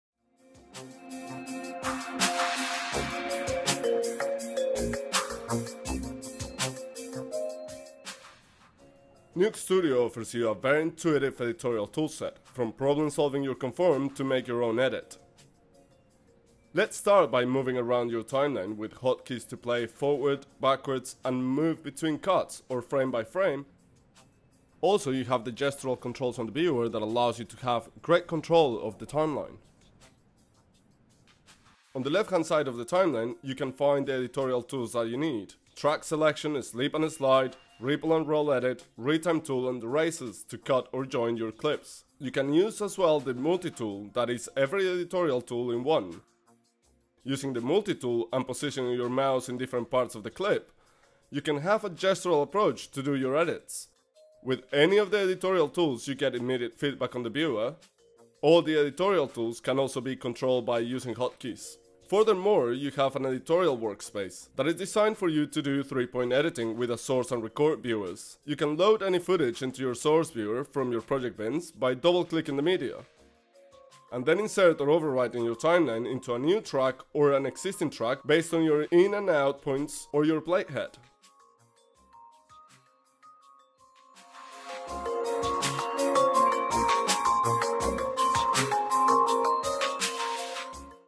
Nuke 9.0 | Editorial Tools
The editorial tools within NUKE STUDIO provide easy to use, familiar functionality for artists accustomed to standard editorial packages - all accessible on the timeline. For more info about the new NUKE range,

Editorial tools at your disposal include select track, slip & slide, roll & ripple, retime, razor, razor all and join. Accessible as either individual tools on the timeline or through a unique multi-tool with NUKE STUDIO, VFX editorial is made fast and simple.